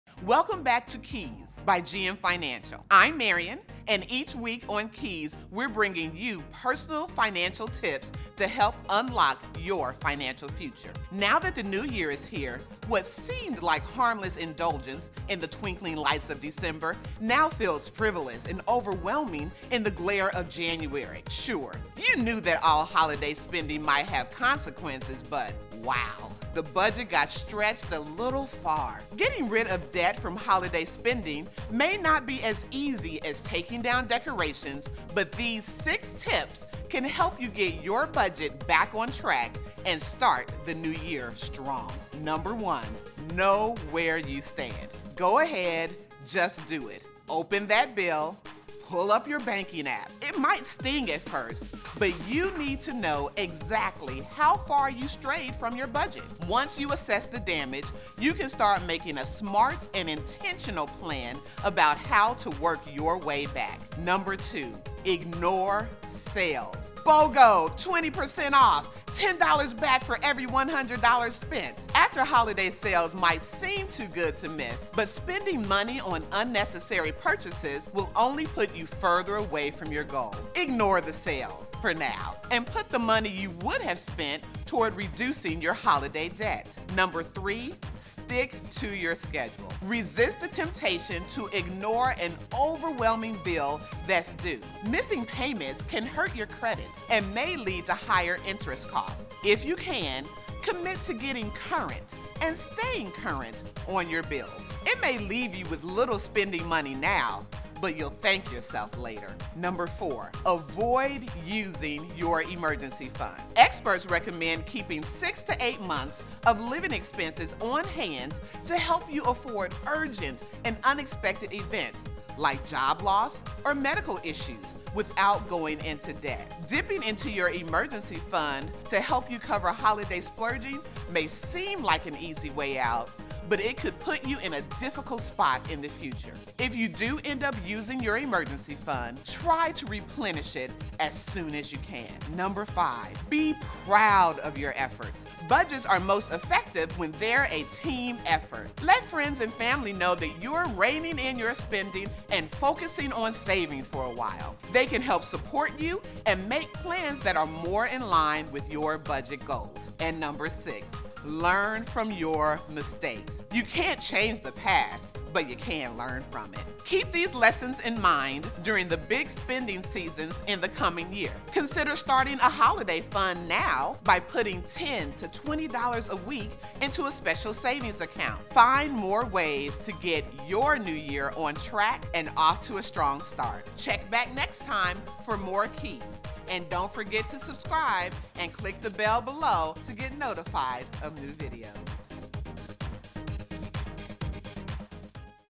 6 Ways To Recover From Holiday Spending | KEYS by GM Financial
New Year, new you. Here's how to help your budget recover and get back on track.